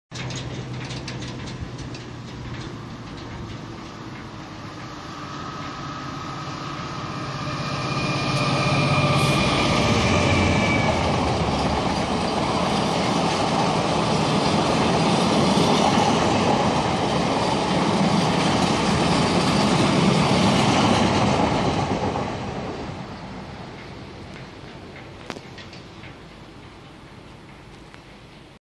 DB Cargo 66013 passing Duffryn
Class 66 Db Cargo  passing with empty wagons /no tones.